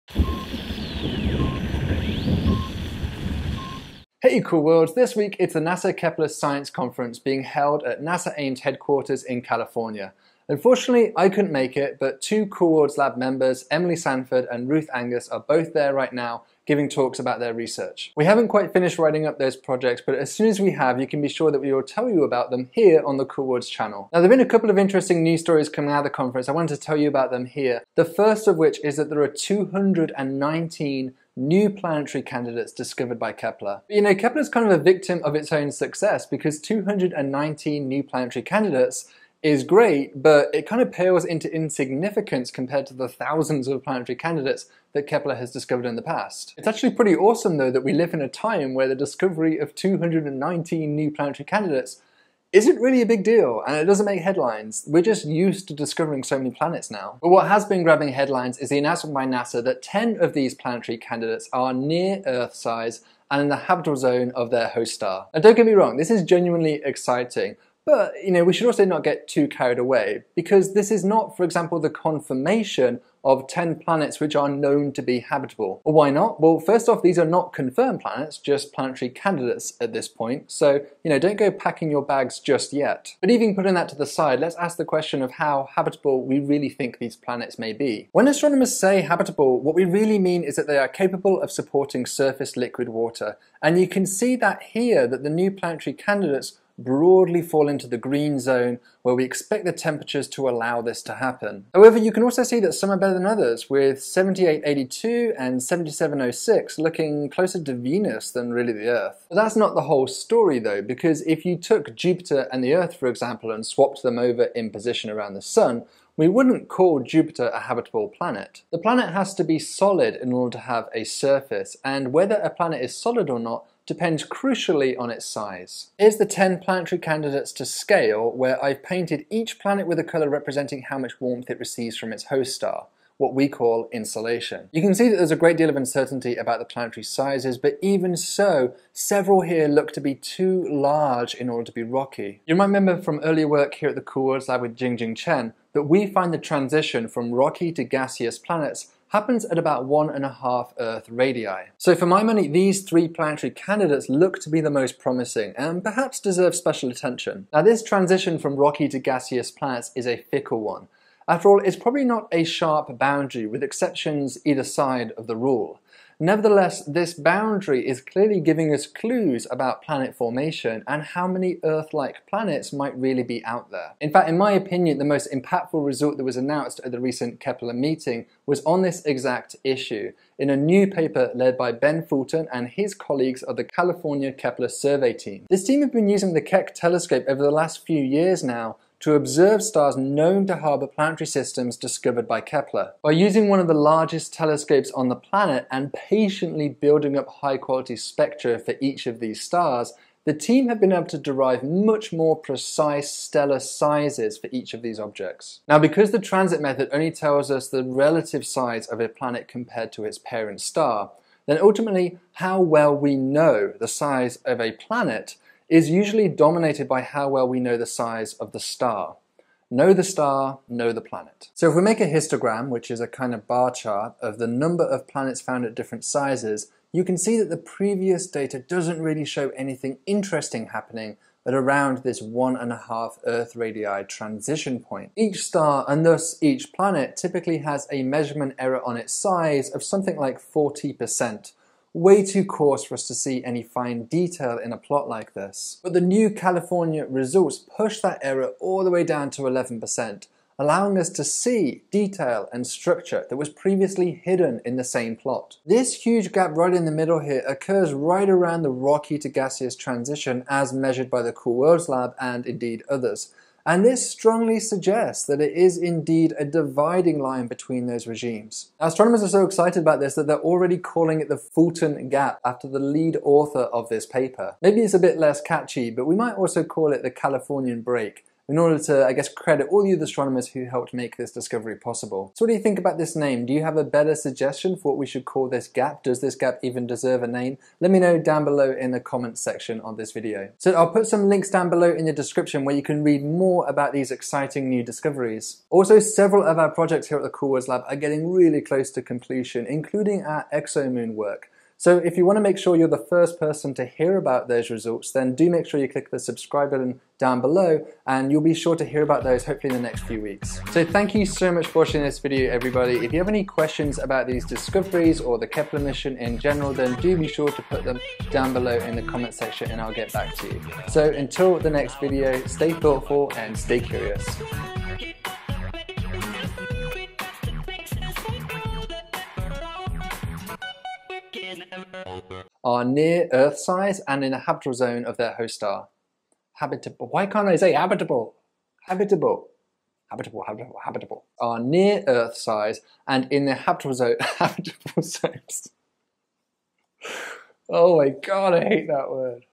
New Discoveries from Kepler
NASA's Kepler Mission is now into's eighth year of discovering new planets. This week, NASA held the fourth Kepler Science Conference at NASA Ames with important discoveries being announced there. Here's a rundown of a couple of the stories making the news and some insights into how these results fit into our emerging picture of exoplanets.

::More about this Video::

► Petigura et al. (2017), "The California-Kepler Survey. I. High Resolution Spectroscopy of 1305 Stars Hosting Kepler Transiting Planets"
► Johnson et al. (2017), "The California-Kepler Survey. II. Precise Physical Properties of 2025 Kepler Planets and Their Host Stars"
► Fulton et al. (2017), "The California-Kepler Survey. III. A Gap in the Radius Distribution of Small Planets"